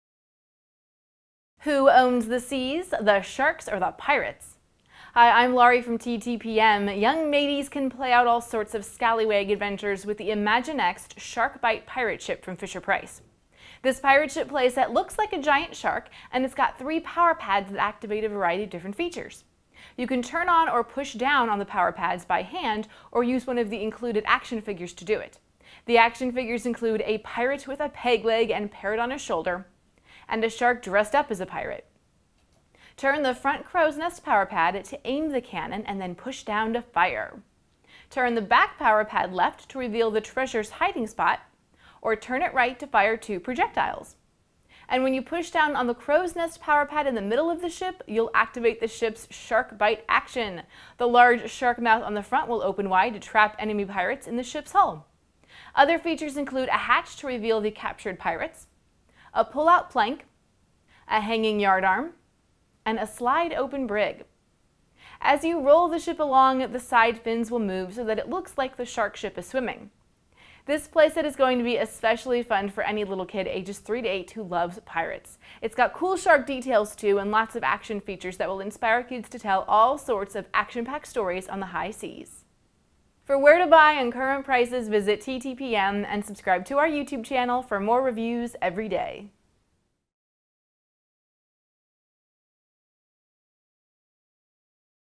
Imaginext Shark Bite Pirate Ship from Fisher-Price
Who owns the seas? It’s up to young mateys to decide with the Imaginext Shark Bite Pirate Ship! This video shows you all the cool features to activate with just a turn or push of the three Power Pads! You get a pirate action figure and a shark action figure that is dressed as a pirate! Shoot the cannons and projectiles, keep the hidden treasure hidden, and make the ship’s shark mouth chomp the enemies! For full review and shopping info

Product Info: Who owns the seas? The sharks or the pirates? What about pirate-sharks? Young mateys can play out all sorts of scallywag adventures with the Imaginext Shark Bite Pirate Ship. This pirate ship playset looks like a giant shark, and it’s got three Power Pads that activate a variety of different features. You can turn or push down on the Power Pads by hand or use one of the two included action figures to do it. The action figures include a pirate with a peg leg and parrot on his shoulder and a shark dressed up as a pirate.

Turn the front crow’s nest Power Pad to aim the cannon, and then push down to fire. Two cannon projectiles are included.

Turn the back Power Pad left to reveal the treasure’s hiding spot, or turn it right to fire two projectiles. Four projectiles are included for these launchers.

And when you push down on the crow’s nest Power Pad in the middle of the ship, you’ll activate the ship’s shark bite action. The large shark mouth on the front will open wide to trap enemy pirates in the ship’s hull.

Other features include a hatch to reveal the captured pirates, a pull-out plank, a hanging yardarm, and a slide-open brig. As you roll the ship along, the side fins will move so that it looks like the shark ship is swimming.